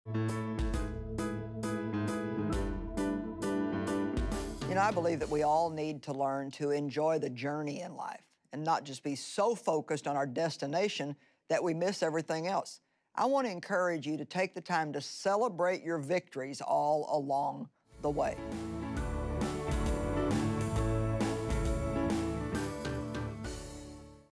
Enjoy Your Journey | Joyce Meyer
Joyce share in this Joyce to Go, why it's so important to take time and do this one simple thing.